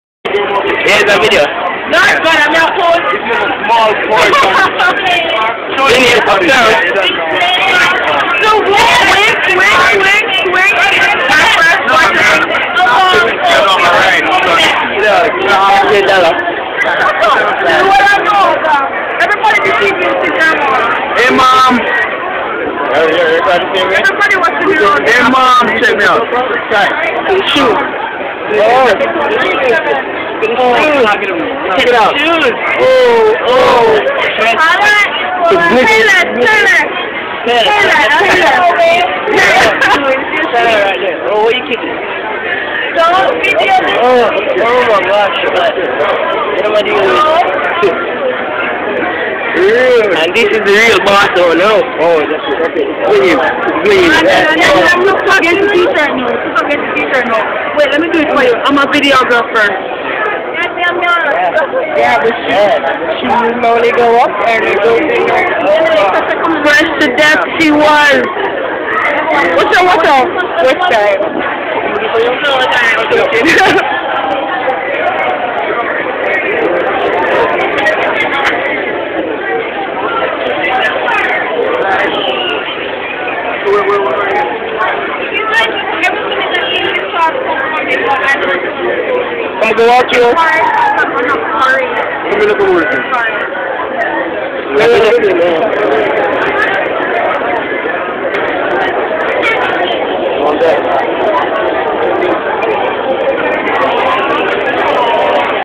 Grand Market 2007 Mandeville
A lot of people hanging out at Mandeville Shopping centre